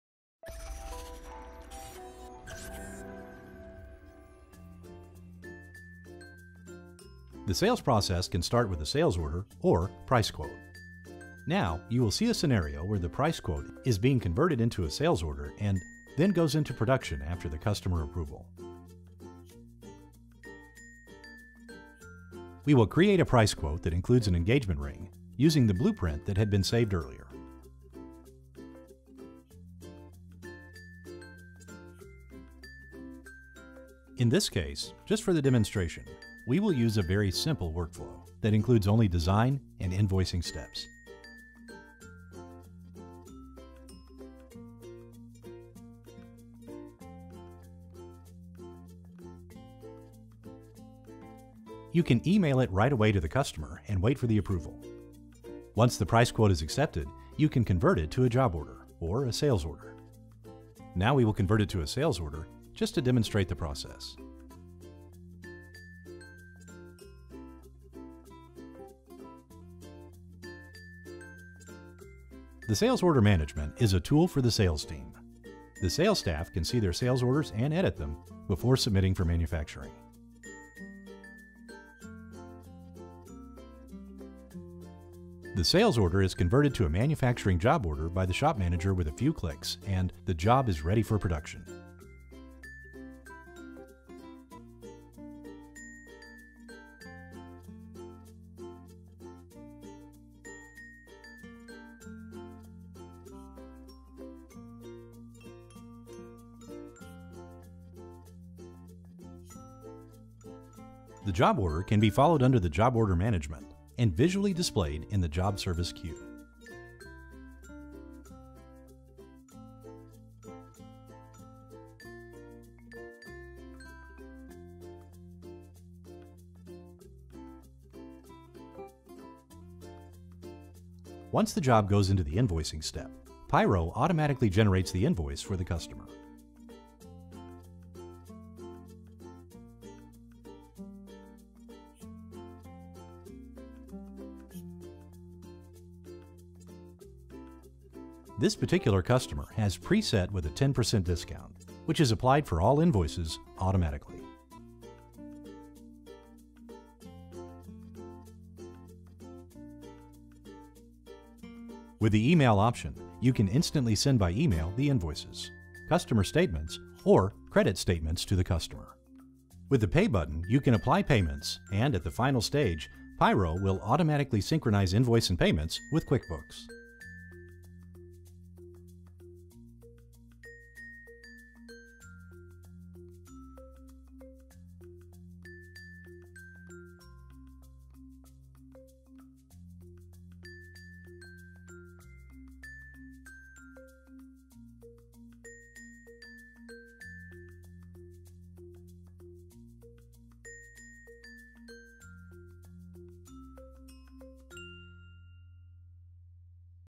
Jewelry software demo - Sales and Accounting features in our software
PIRO is an innovative jewelry software specifically developed for the jewelry industry, allowing seamless management of manufacturing, retail and wholesale operations by combining functionality, ease of use, and flexibility. PIRO Retail is a next-generation POS system for manufacturing jewelers.

In this demo video we explain about the Sales and Accounting features in our jewelry software:
*PIRO will automate your sales process, allowing you to create and process sales orders with just a few clicks. Numerous tools are available to streamline the sales from price quoting to invoicing.*

0:00 - Intro
0:06 - Price quotes is converted in sales order
1:15 - Sales order management
1:50 - Job order management
2:15 - Invoicing
2:48 - Payment